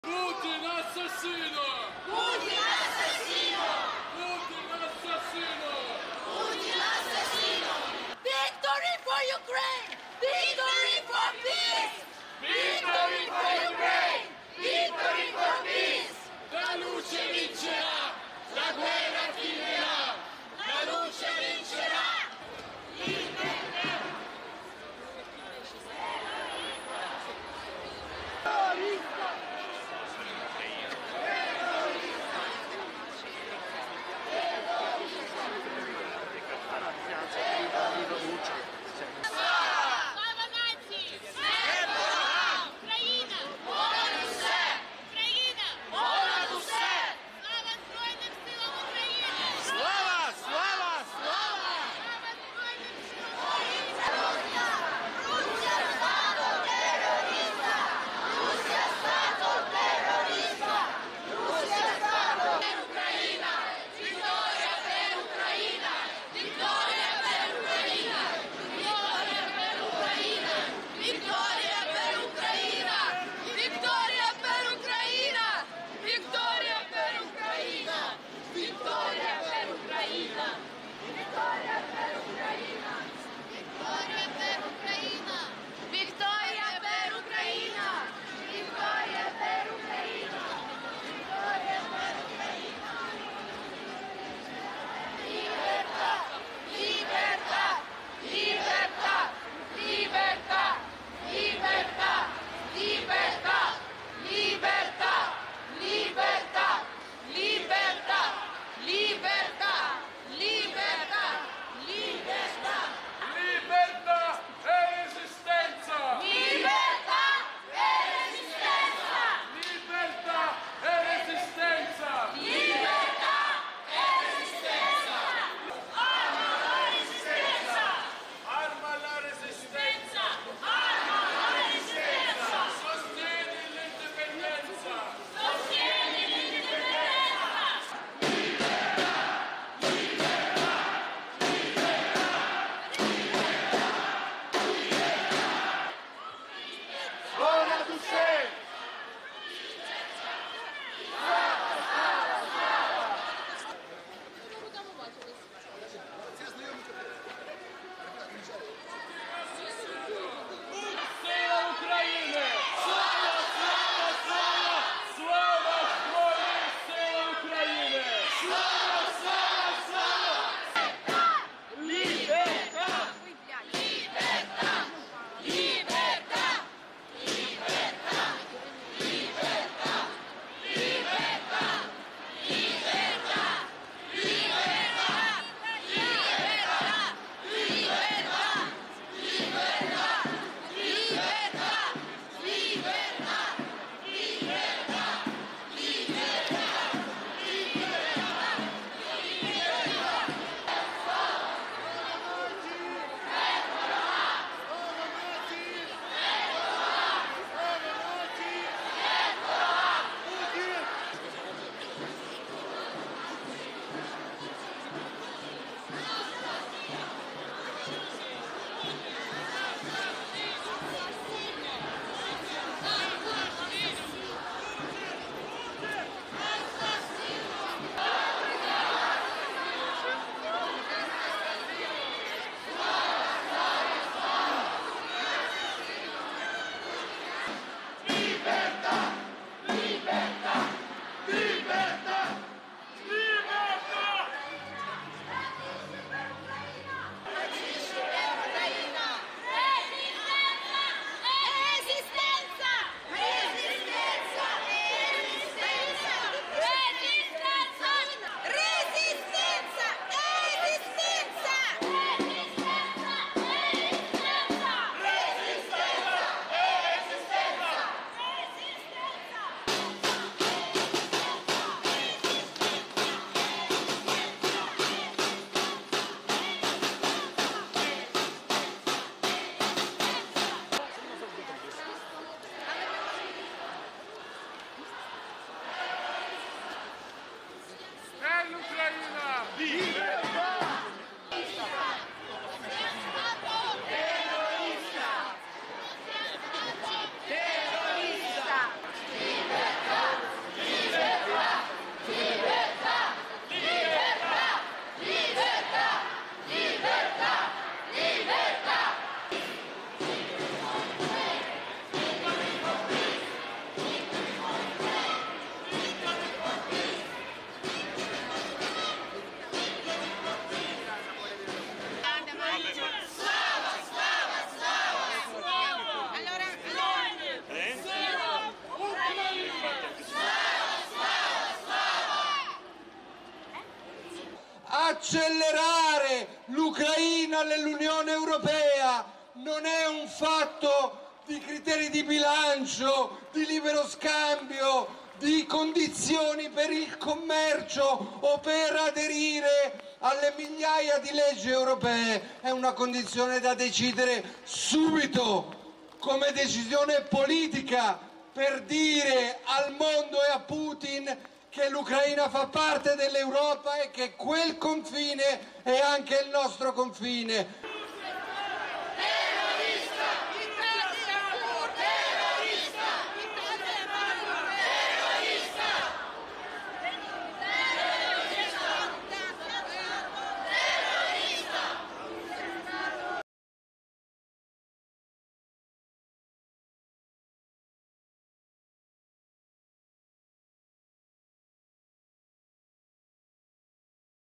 Milano, comunità ucraina in corteo a due anni dall'invasione russa
Il corteo della comunità ucraina milanese è giunto a destinazione in Piazza San Babila. Tra gli interventi, quello del politico e attivista per i diritti civili e sociali Marco Cappato. La manifestazione si svolge nel secondo anniversario dell'invasione russa dell'Ucraina.